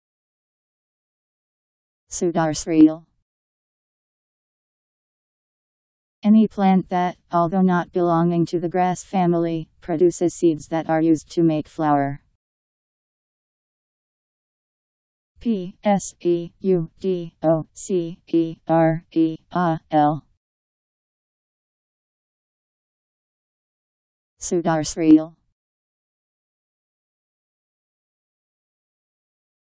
Pseudocereal Meaning
Video shows what pseudocereal means. any plant that, although not belonging to the grass family, produces seeds that are used to make flour.  Pseudocereal Meaning. How to pronounce, definition audio dictionary. How to say pseudocereal. Powered by MaryTTS, Wiktionary